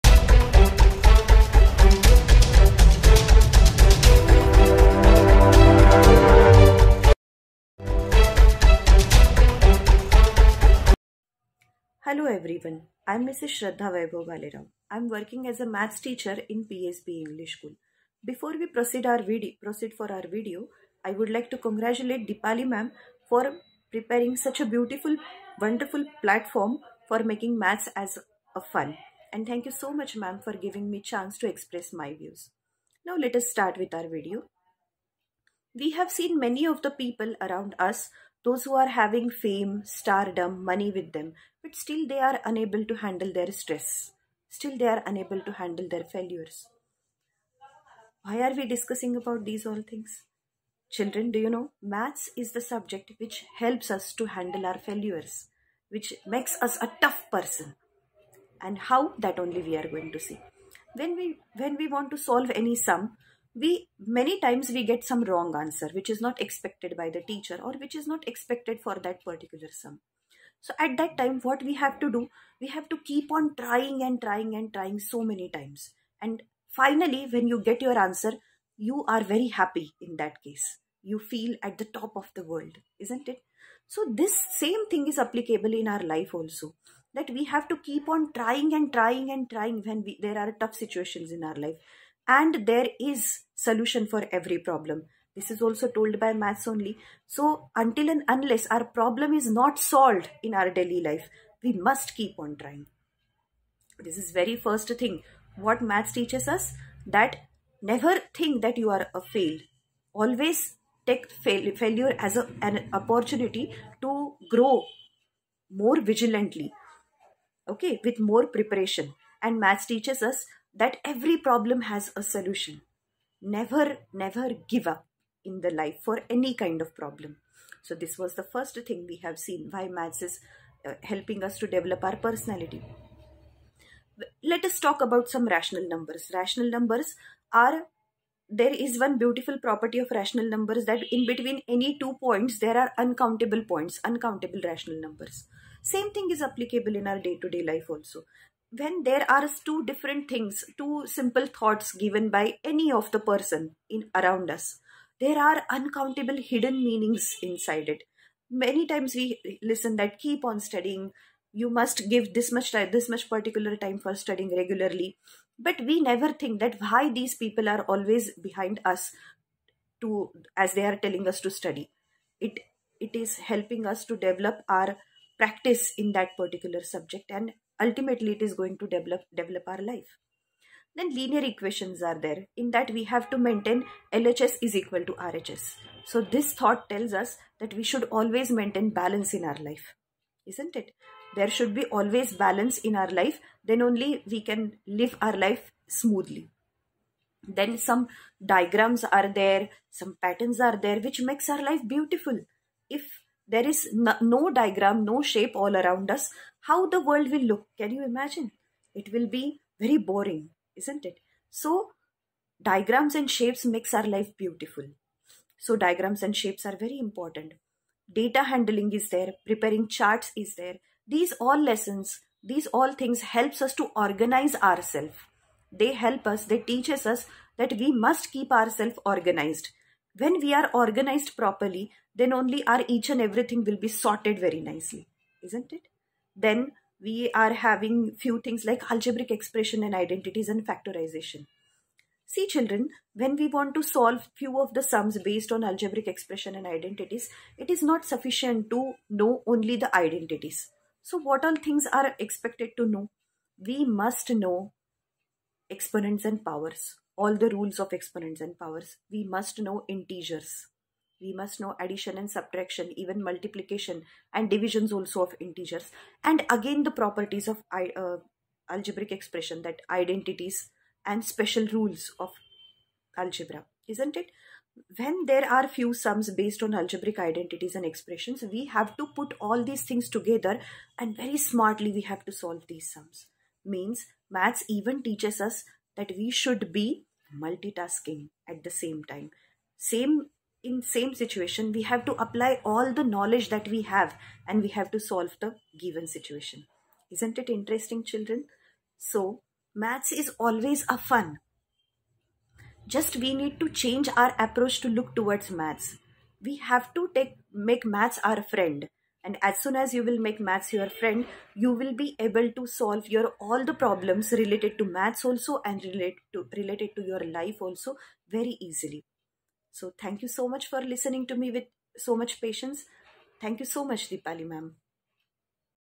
Maths Talk Show | Fun Learn | Ft. Shraddha Bhalerao - mathematics and personality development
Maths talk show , a platform for students , teachers , parents and everyone who like to share their views about mathematics
maths talk show a little step towards increasing awareness of mathematics
in Today episode let's meet Shraddha mam who help us to  understand the role of mathematics in Personality development.